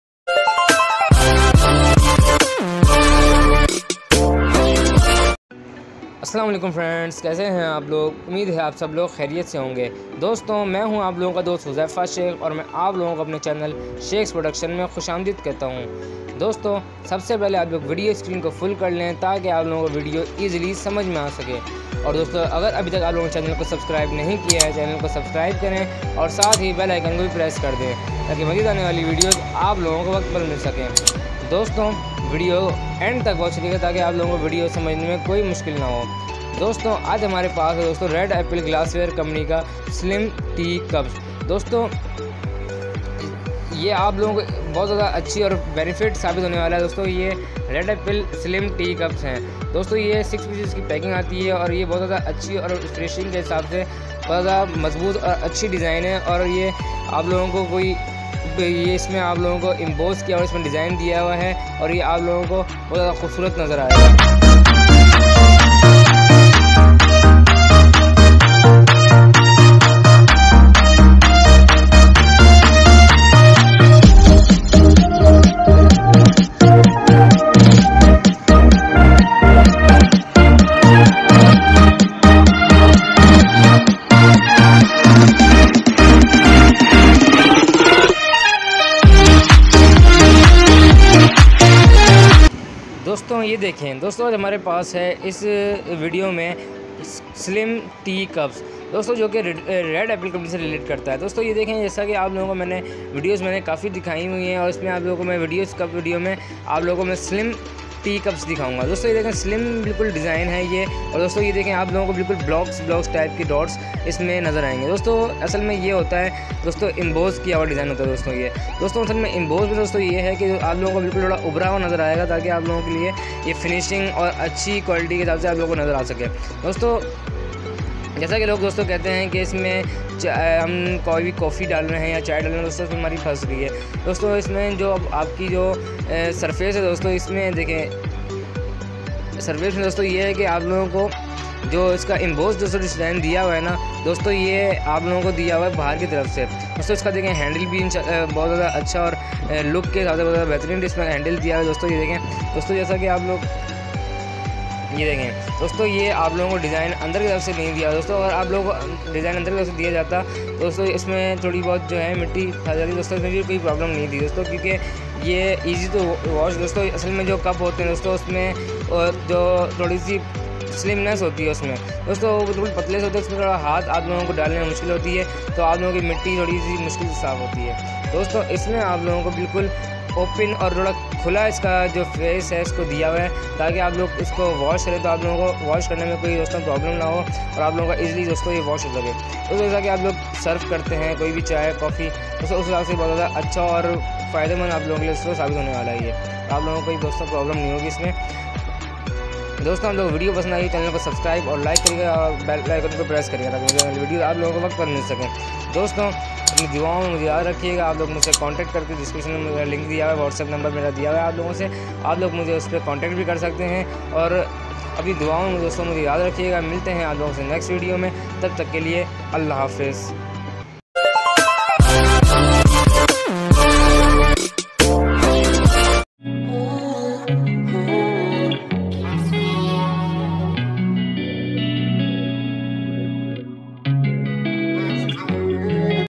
Slim Tea Cups || Latest Design || Red Apple Company
Assalam-u-Alaikum
I'm huzaifa shaikh from gul centre
In this video I have latest design of slim tea cups It is amazing and beautiful design for multipurpose use
also finishing is very beautiful
thanks
Business name (Gul Centre)
Channel name (Shaikh's Production)
Business address (Sukkur Township)